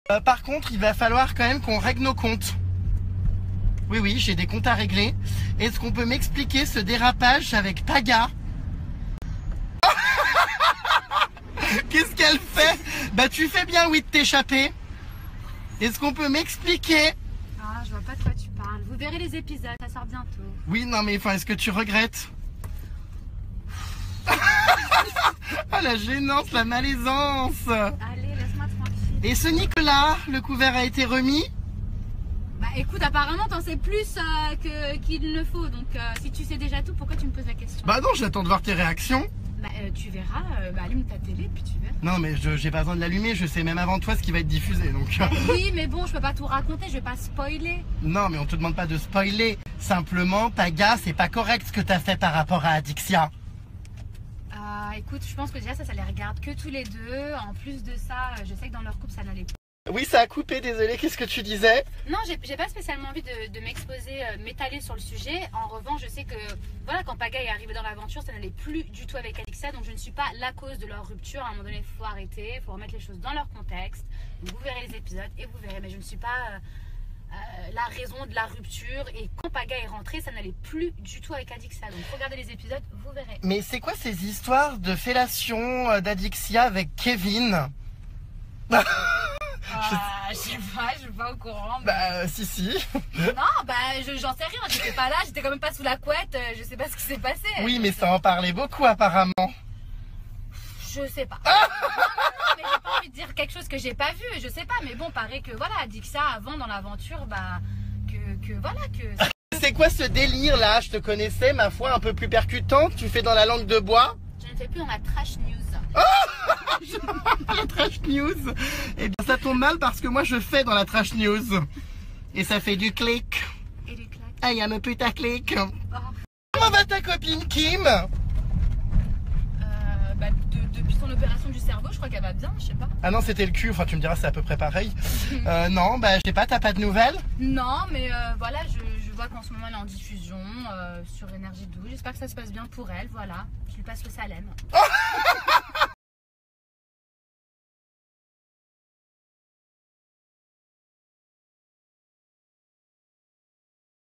Milla Jasmine RESPONSABLE de la RUPTURE entre Adixia et Paga ? Elle RÉAGIT sur Snapchat
Milla Jasmine (les marseillais vs le reste de monde) RESPONSABLE de la RUPTURE entre Adixia et Paga ? Elle RÉAGIT sur Snapchat